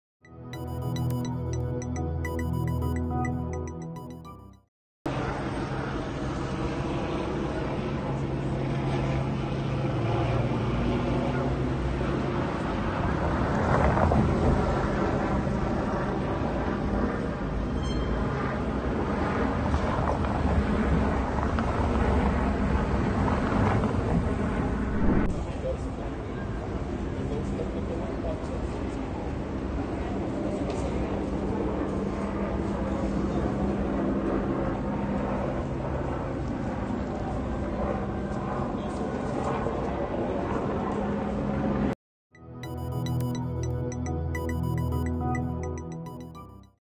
Lula chega ao CCBB para anunciar novos ministros
O presidente eleito Luiz Inácio Lula da Silva chegou na manhã desta 5ª feira (22.dez.2022) ao CCBB (Centro Cultural Banco do Brasil), em Brasília, para anunciar os nomes dos novos ministros que irão compor sua equipe a partir de 2023.